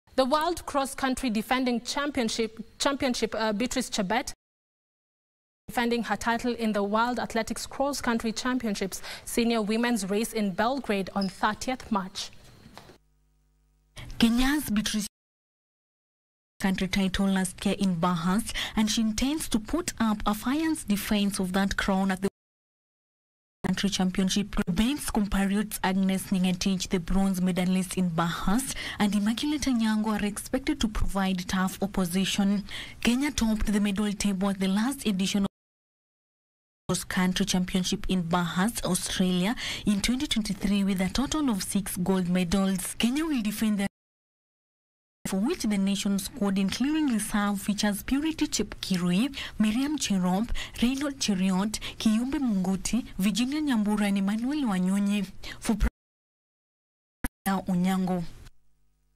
Beatrice Chebet to defend her title in World Athletics Cross Country Championships in Belgrade
World cross country defending Championship Beatrice Chebet is looking forward to defending her title in the World Athletics Cross Country Championships senior women’s race in Belgrade on 30 March.